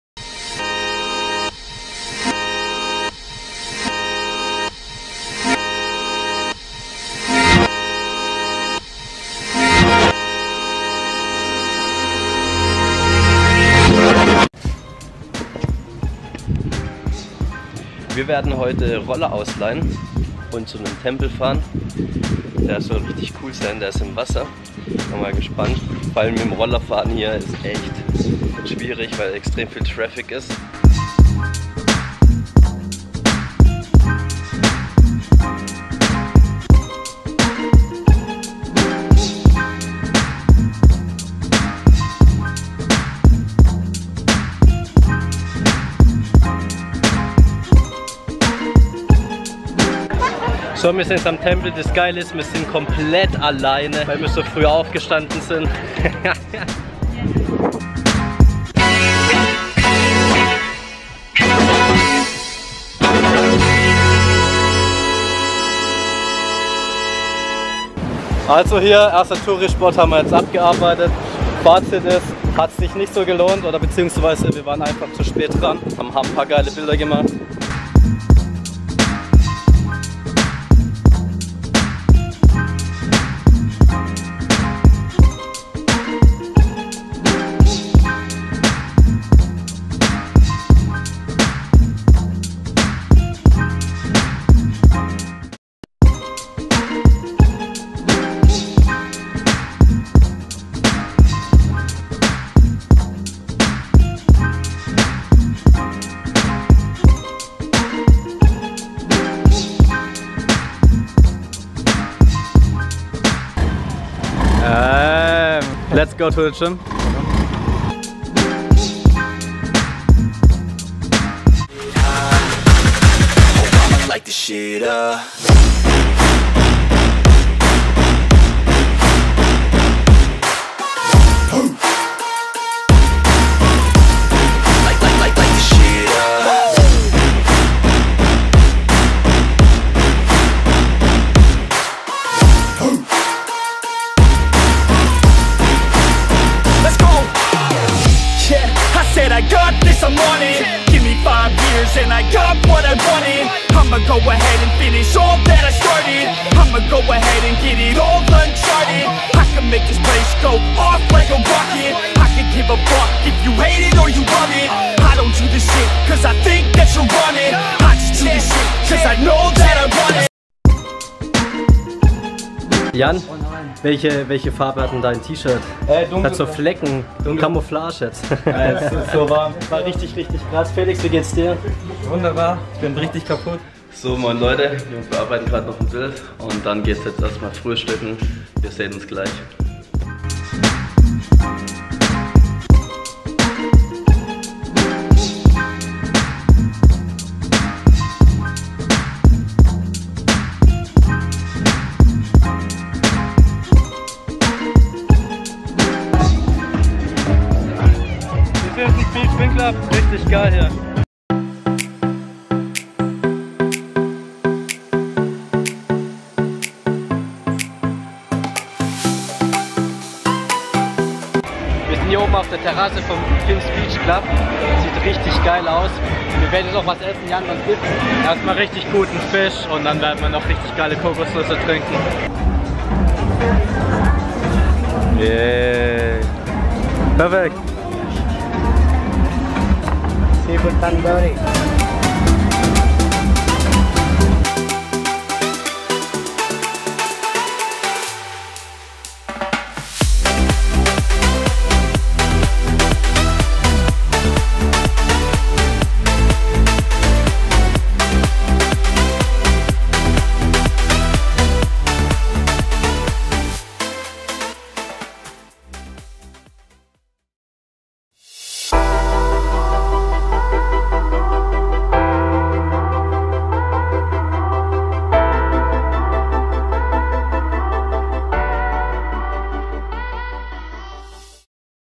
VLOG 2 - WHY WE LOVE CANGGU
Wir hatten zwei tolle Tage in Canggu (Bali). Mit dem Scooter sind wir zum Tanah Lot Temple gefahren. In den Städten ist schon viel Traffic. Wir hatten zu dritt eine gesamte Villa für 60€ die Nacht. Canggu ist sehr schön! Seht selbst :-) VLOG #3 geht über unsere Zeit in Ubud.

Kamara: Canon GX7 & Canon 6D MarkII